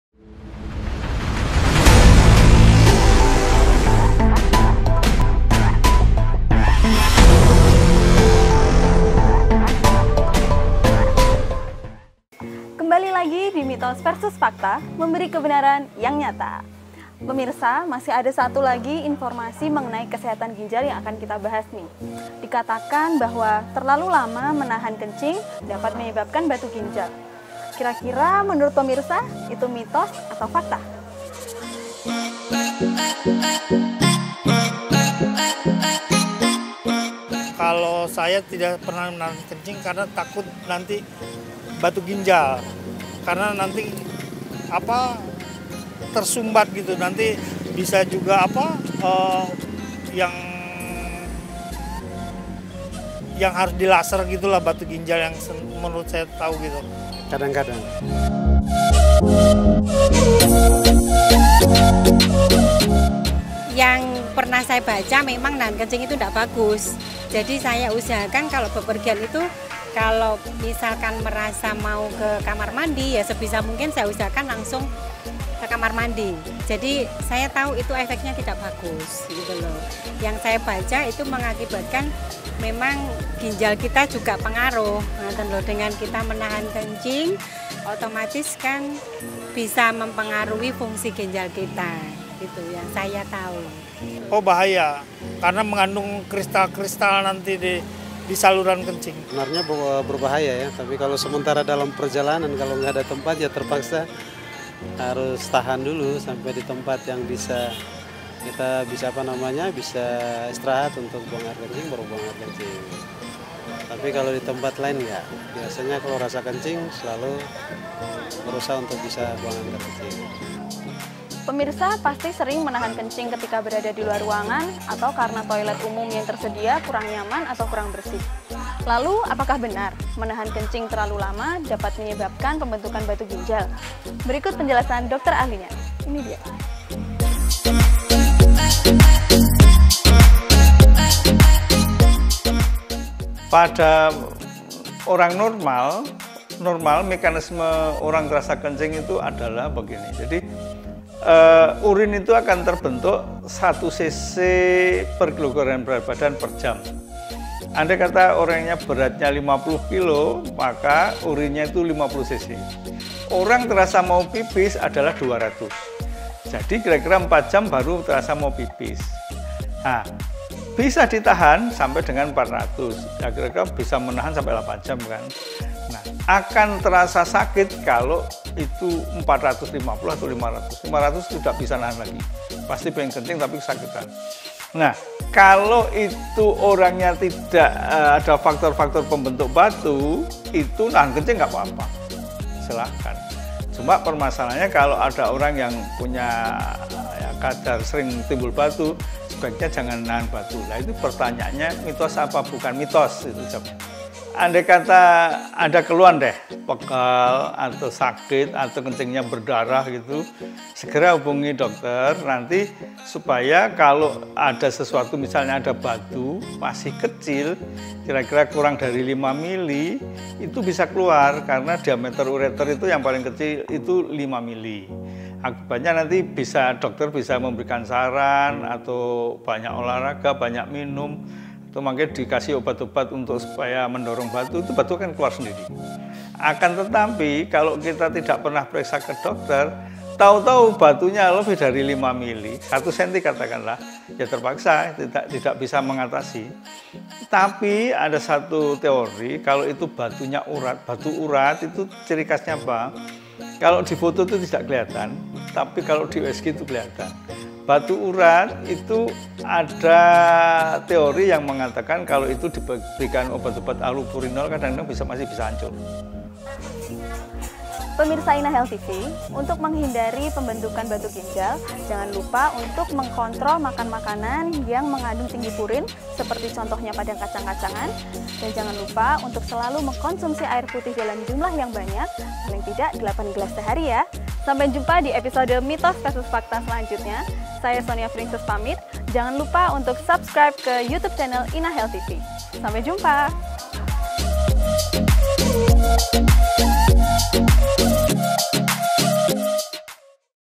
MENAHAN KENCING MEMICU BATU GINJAL ? - MITOS VS FAKTA
Sering kita dengar saran dari orang terdekat yang meminta kita untuk segera buang air kecil  jika sudah memiliki keinginan untuk melakukannya. Jika kita terbiasa menahannya karena ingin menyelesaikan berbagai hal  terlebih dahulu, dikhawatirkan kita akan
mengalami masalah batu ginjal.  Sebenarnya, apakah kepercayaan ini benar adanya?

Apa Benar menahan kencing memicu bantu ginjal ,, temukan faktanya...

Inahealth - Trustedhealthchannel
"Mengupas masalah kesehatan secara ringan & menarik langsung dari ahlinya"